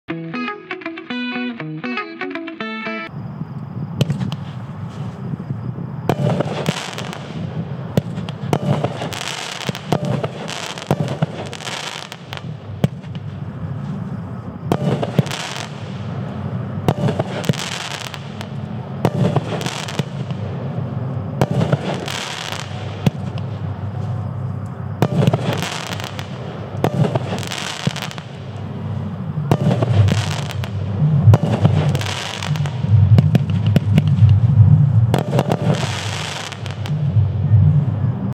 Super Stock Car Brothers 350 Gram Cakes
This 350 gram monster is high on horsepower.  The green flag falls on red, yellow, green and white tails to palm and crackling. Duration: 35 secs. 12 per case.
Approximate height reflects final camera position.